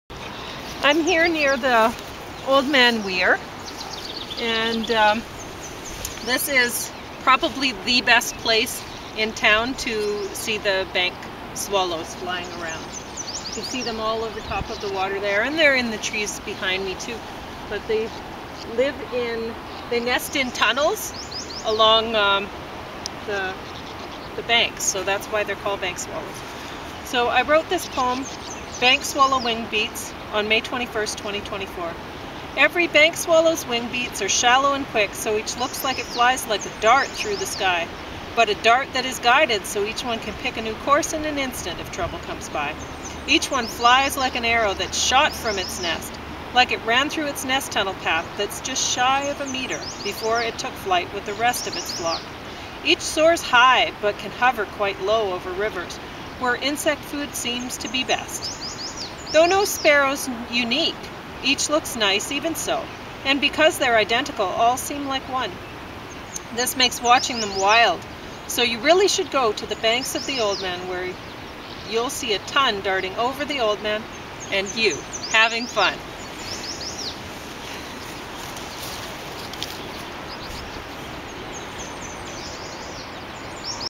Bank Sparrow Wingbeats (poem by Pauline Bell) read on the Oldman Riverbank near the Lethbridge Weir.
May 22, 2024. 9:32 AM. Botterill Bottom Park, Lethbridge, Alberta. There were quite a few Pelicans at the weir this morning.

If you enjoyed this video, you’ll like my blog even more. Copy and paste the following address (to my blog) into a search bar.